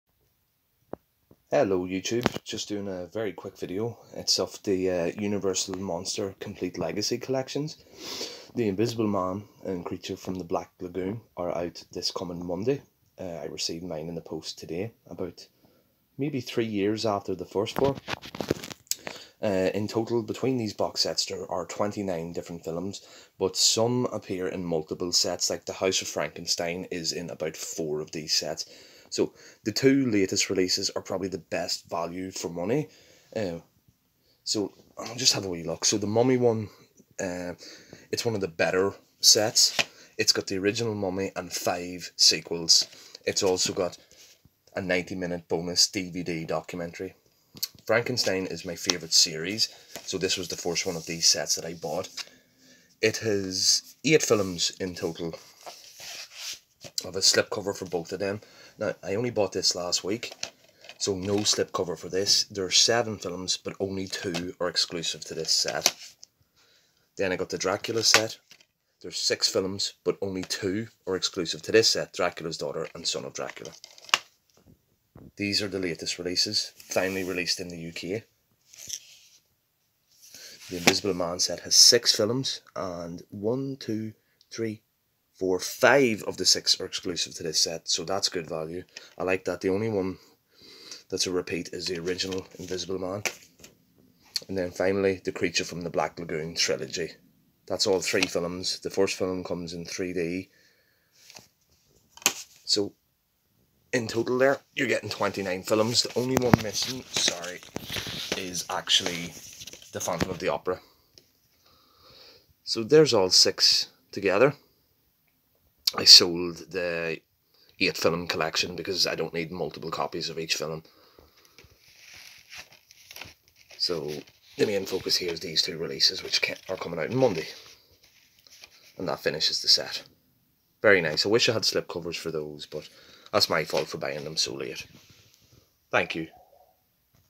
Universal Monsters Complete Legacy Collection Blu Ray - UK Releases - 2019 Update
29 films in total - all six Universal Monster Legacy Collections now including Creature From The Black Lagoon and The Invisible Man!
Come chat on Twitter! Pop by and see my posts on Instagram!